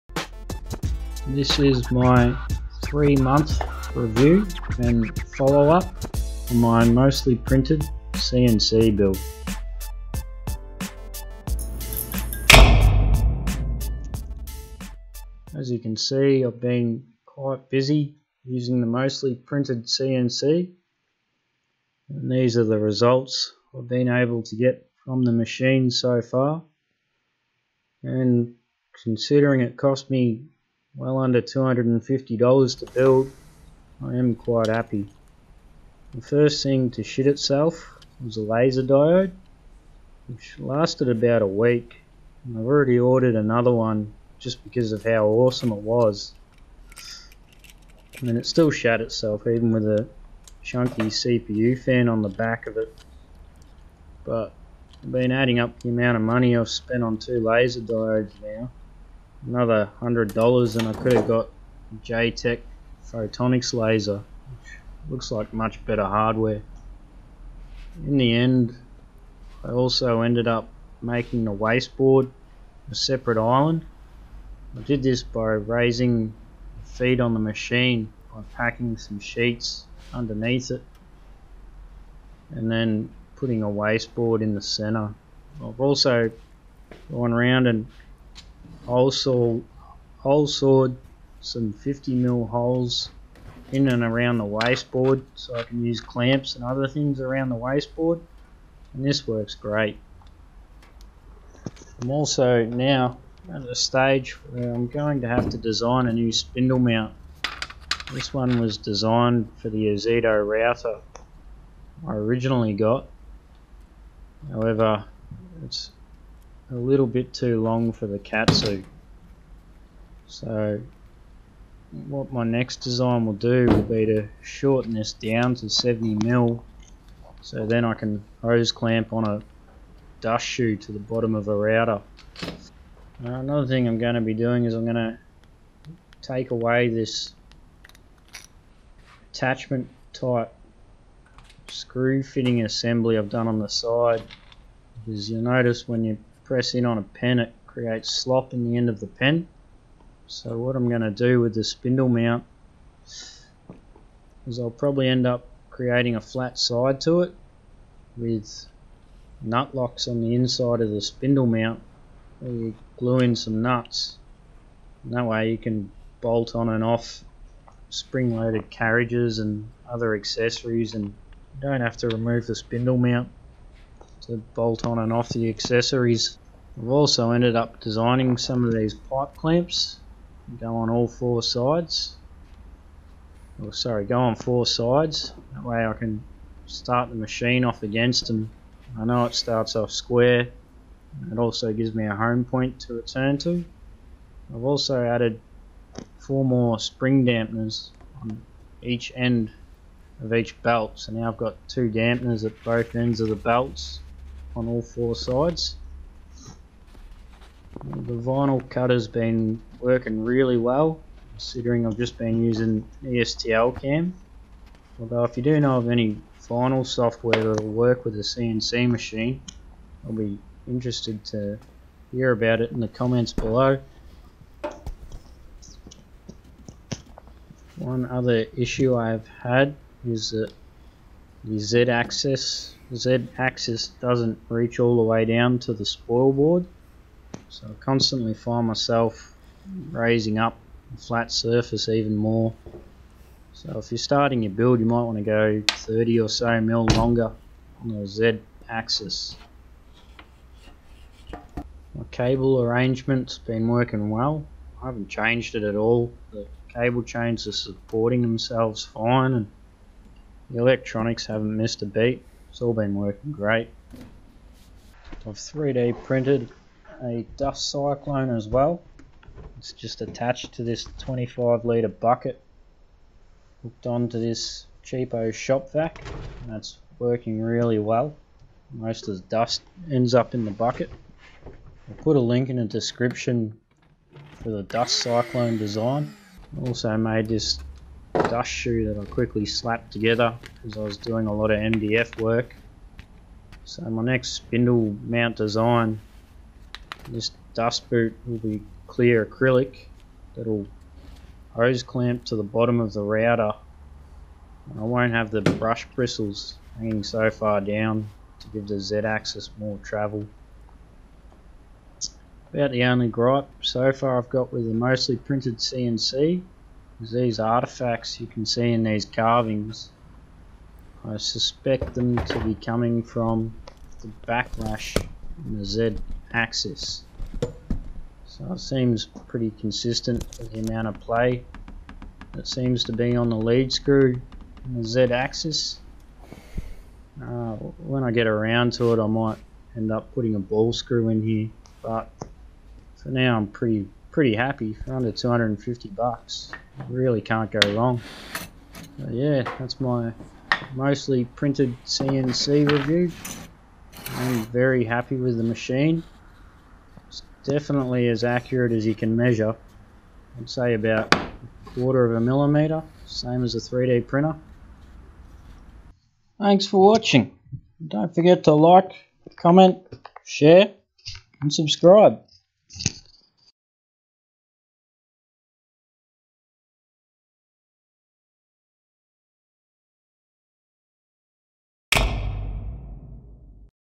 mpcnc 3 month review
in this video i go over how my mostly printed cnc is going and the changes i have and am going to make.

for the plans go to

dust cyclone design

all my STL files are on my website and at my Mini Factory